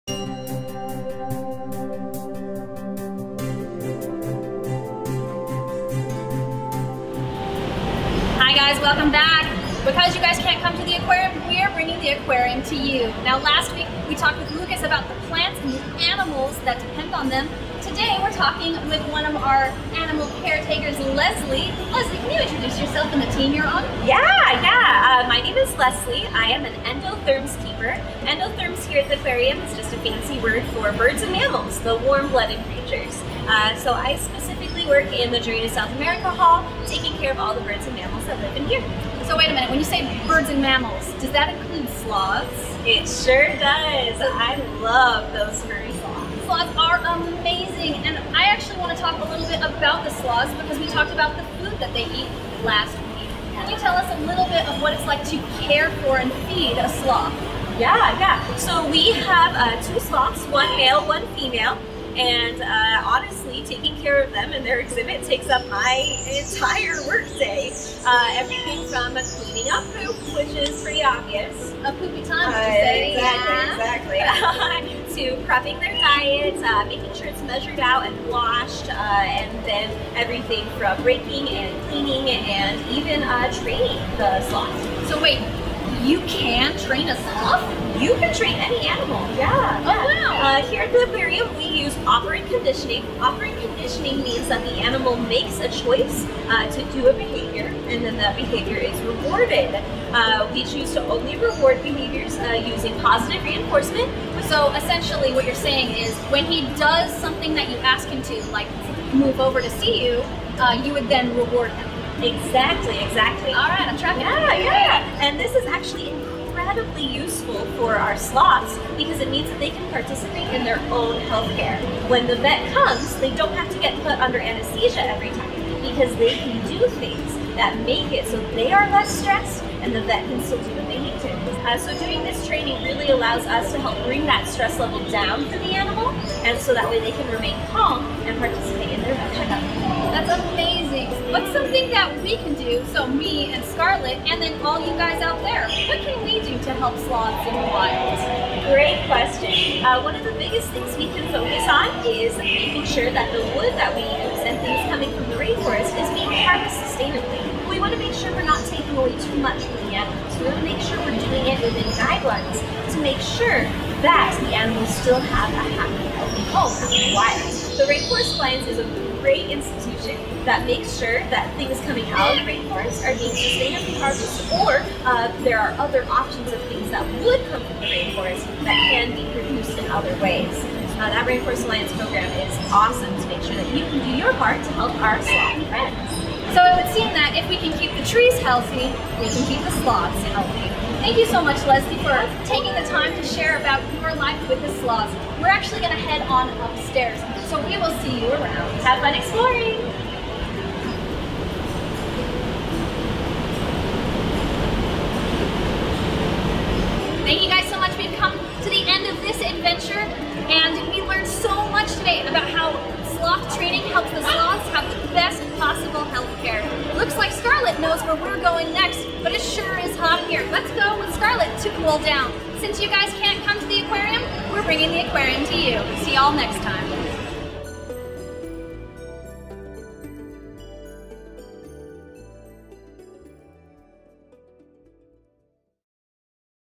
Scarlet's Adventure Episode 2: Hangin' with Sloths
Scarlet is continuing her adventure in Journey to South America, and in this episode, she met with Lesley to learn about Sloths.
Loveland Living Planet Aquarium is bringing the Aquarium to YOU with exciting videos that you can enjoy at home with your kids.